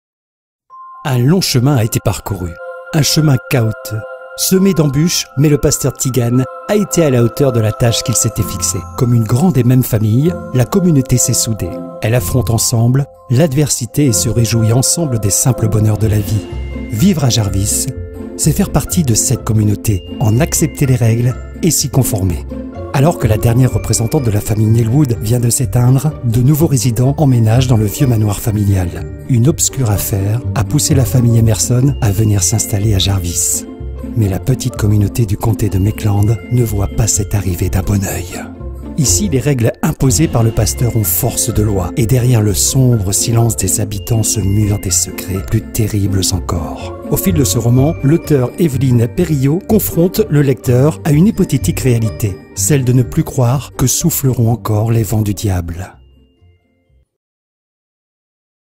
Les vents du Diable
Un long chemin a été parcouru, un chemin cahoteux, semé d'embûches mais le pasteur Tigan a été à la hauteur de la tâche qu'il s'était fixée.
Comme une grande et même famille, la communauté s'est soudée. Elle affronte ensemble l'adversité et se réjouit ensemble des simples bonheurs de la vie. Vivre à Jarvis, c'est faire partie de cette communauté, en accepter les règles et s'y conformer...
Alors que la dernière représentante de la famille Nelwood vient de s'éteindre, de nouveaux résidents emménagent dans le vieux manoir familial. Une obscure affaire a poussé la famille Emerson à venir s'installer à Jarvis, mais la petite communauté du comté de Mechland ne voit pas cette arrivée d'un bon œil.
Ici, les règles imposées par le pasteur ont force de loi et derrière le sombre silence des habitants se murent des secrets plus terribles encore.
Au fil de ce roman, l'auteur, Evelyne Peyrillaud, confronte le lecteur à une hypothétique réalité, celle de ne plus croire que souffleront encore les vents du Diable.